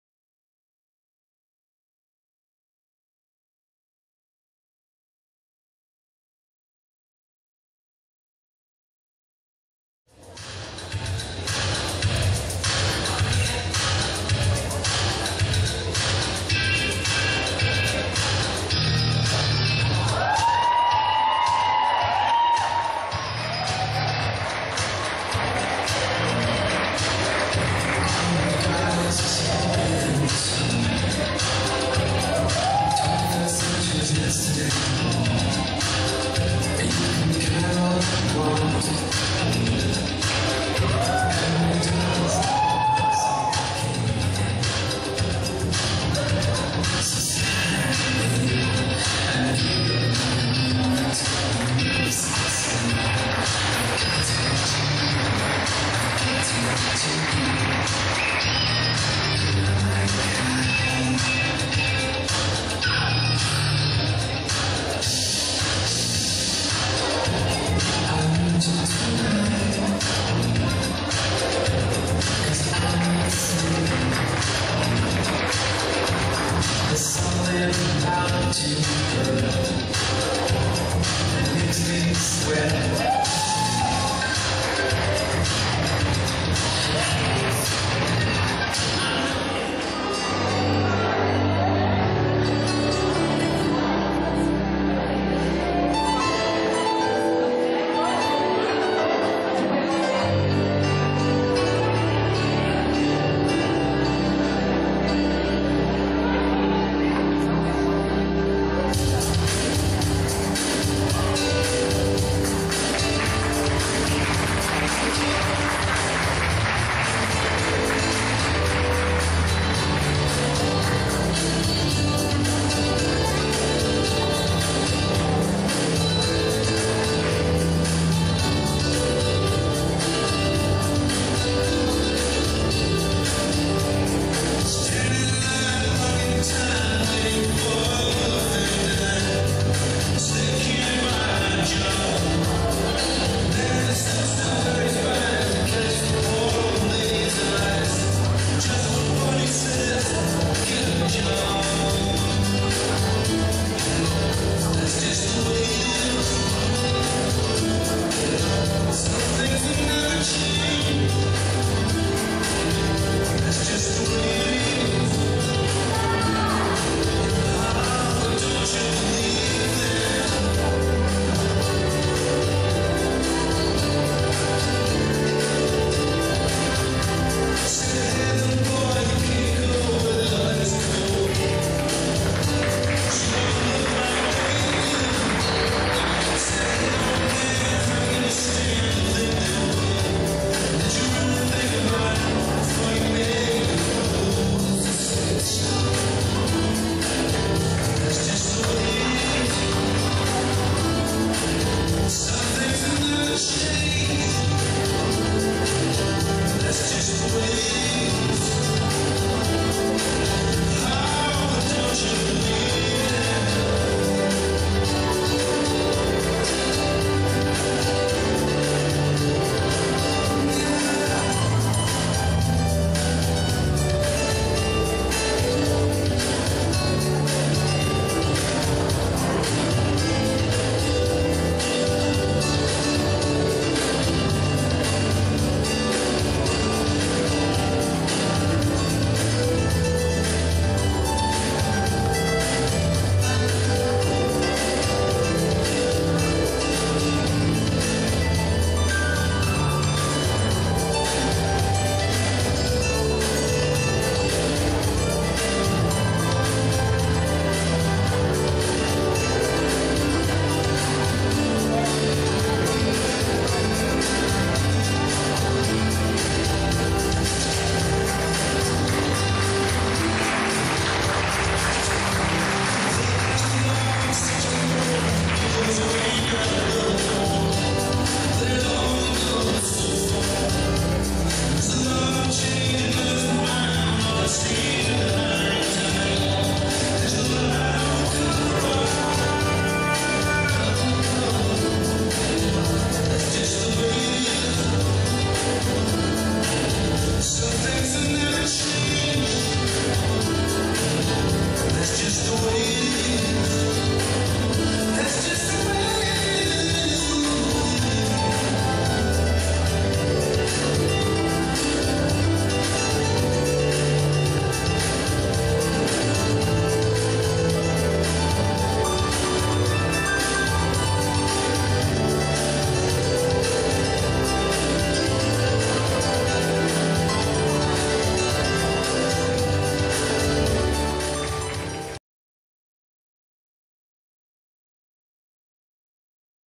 Desfile Traje Esportivo - Bio Models 2016
Desfile do Curso Bio Models, realzado no Centro comunitário de Casa Branca em 13/05/2016. Traje: Esportivo.